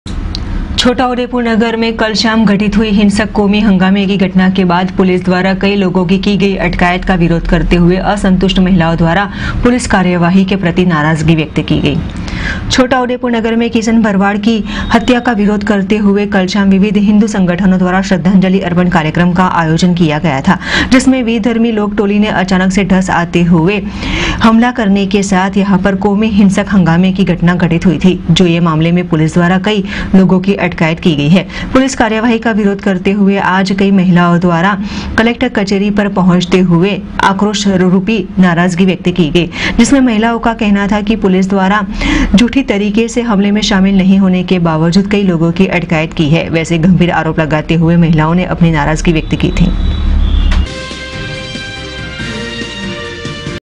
ibn news vadodara
छोटा उदेपुर नगर में हिंसक दंगे की घटना के विरोध में पुलिस कार्यवाही से असंतुष्ट महिलाओंने जताया आक्रोश